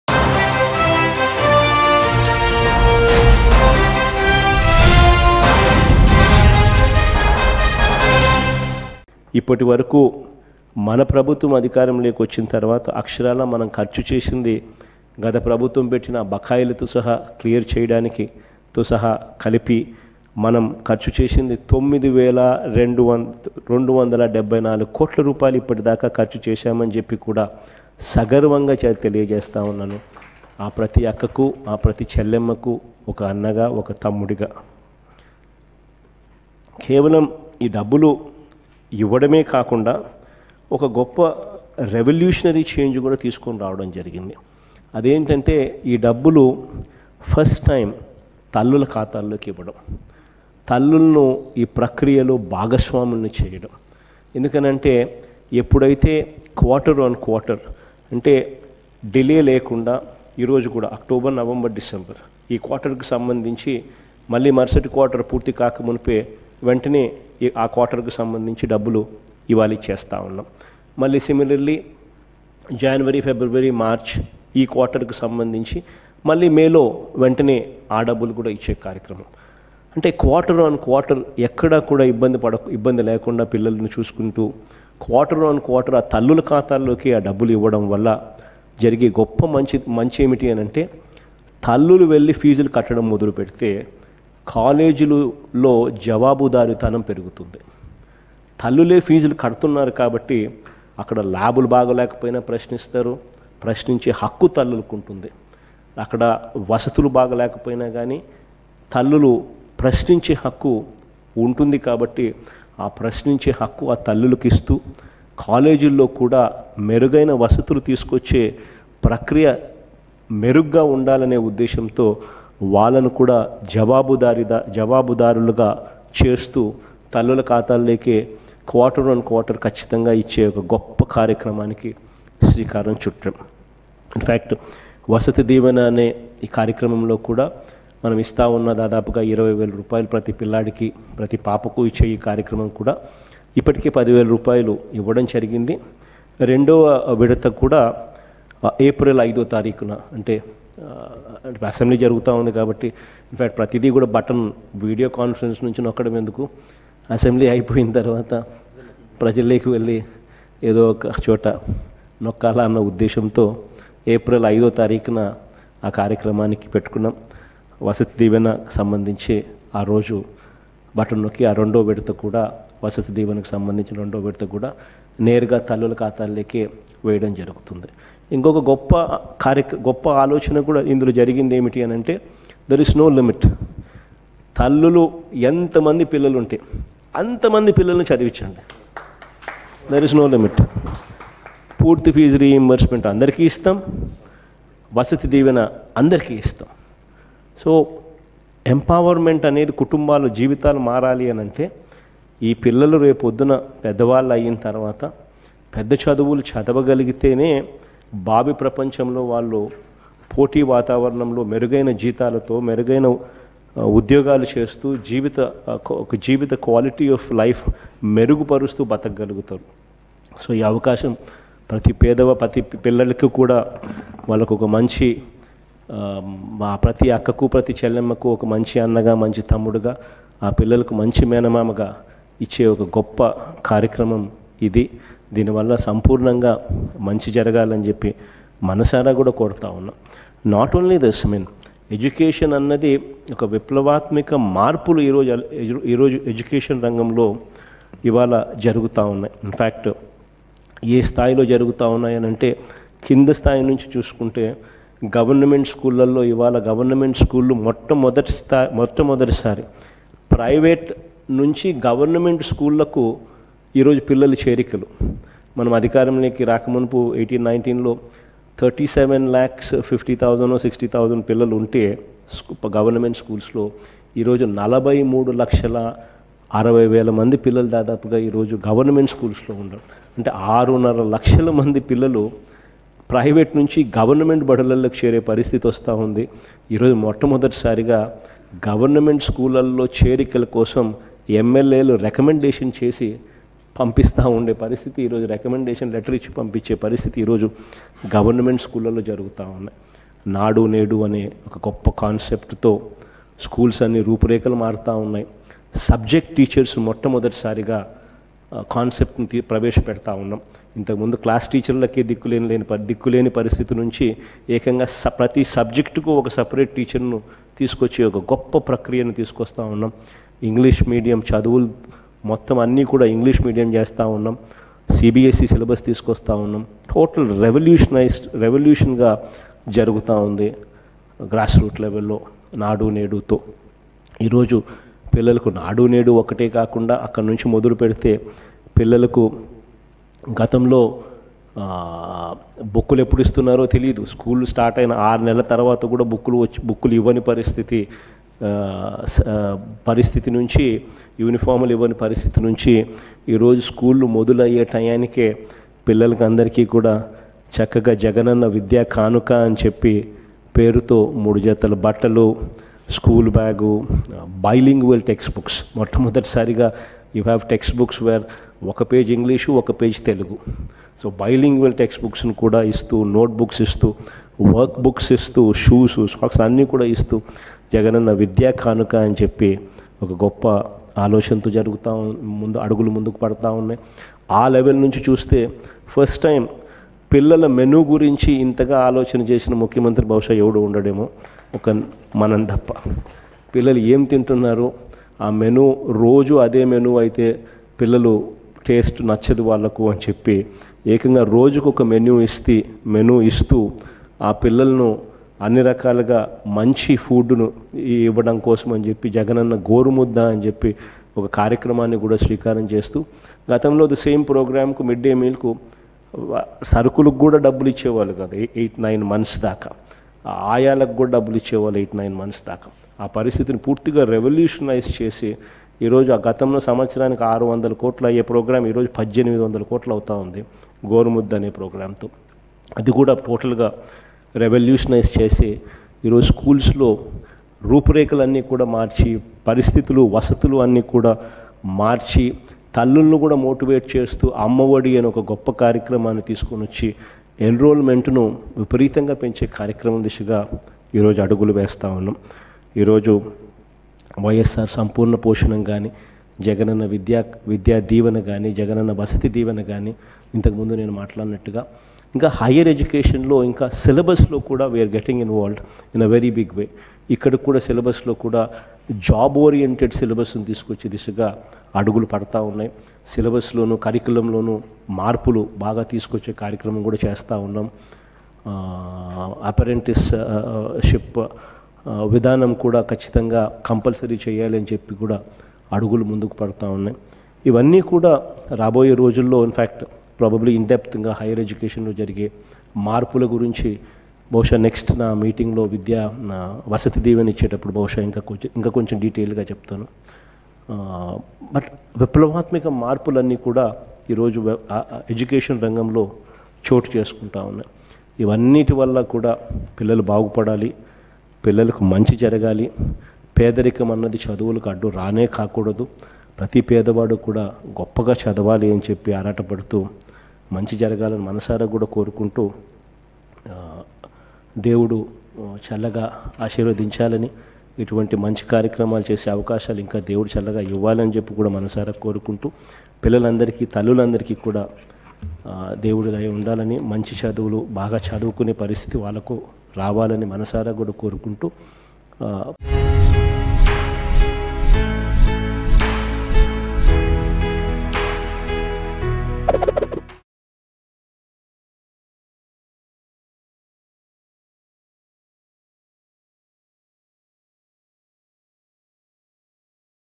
AP CM YS Jagan Serious Comments On Opposition Parties | YCP Vs TDP | AP Politics | Mango News
AP CM YS Jagan Serious Comments On Opposition Parties.